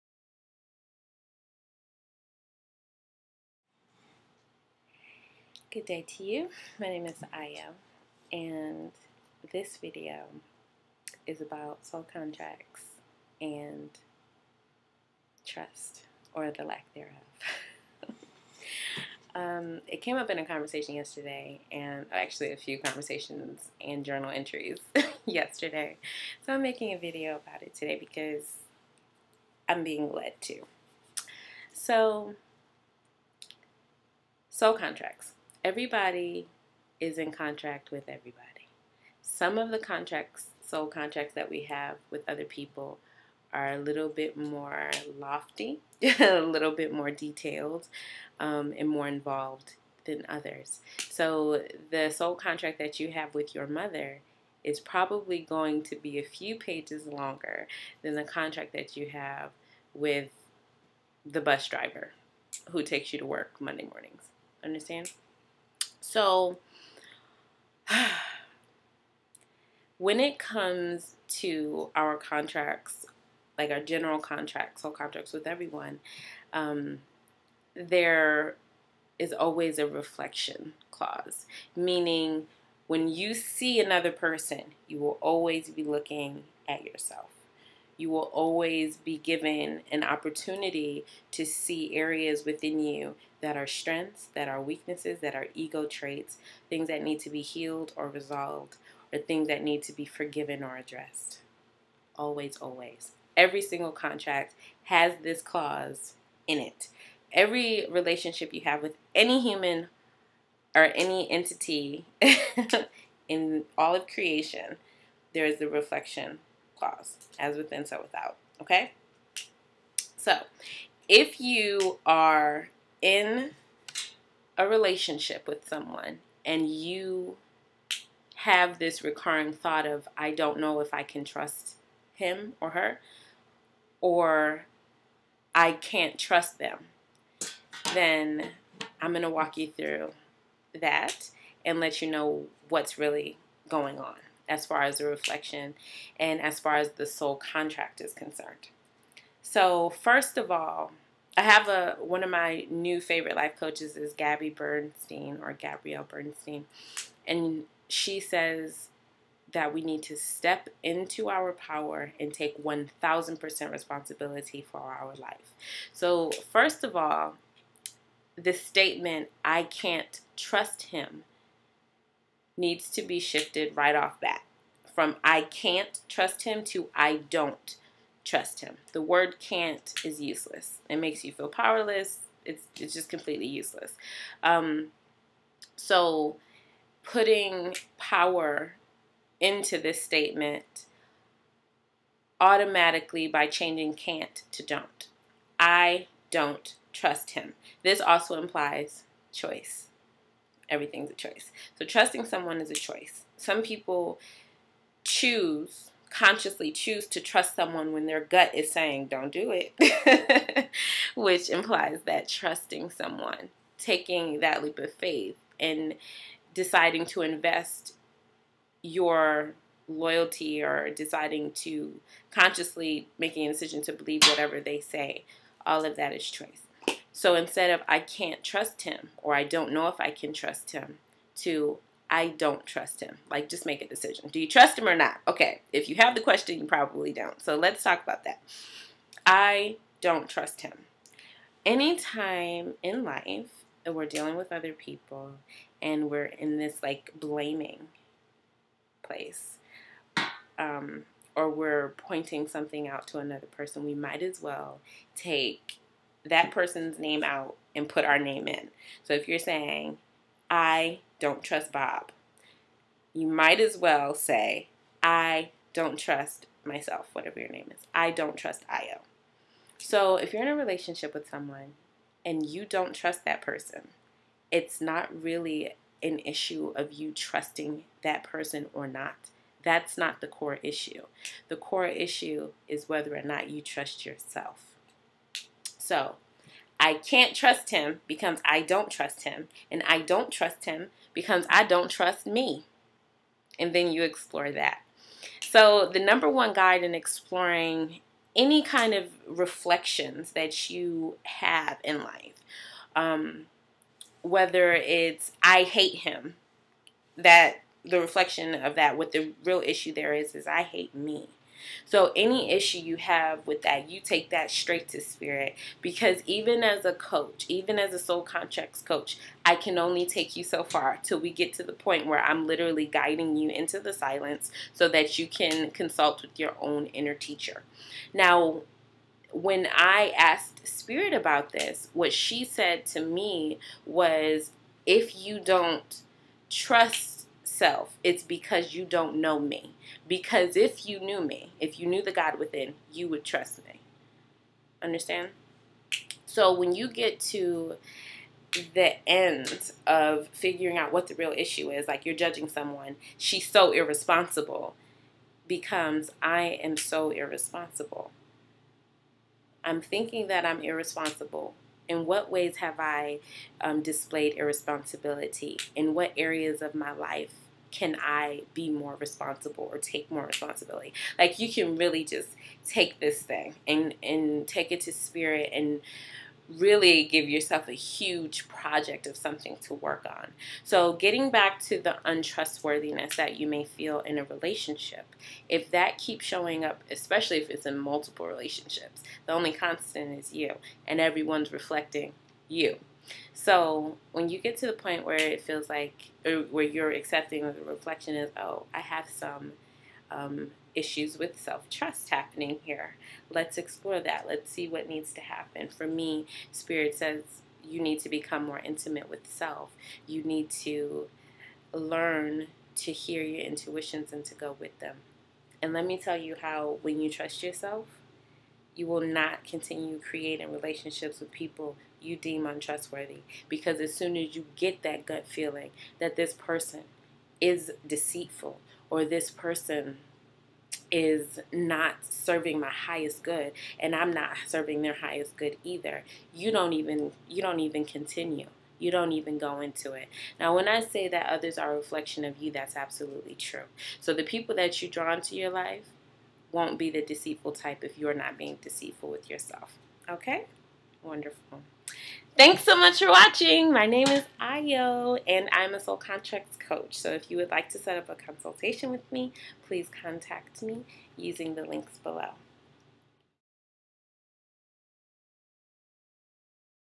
Soul Contracts & The Reflections Clause
If you don't know what a soul contract is this video is for you. Also, if you're in a relationship with someone who is "untrustworthy" this video is specifically for you, my friend. Enjoy!

Loving you,
ayo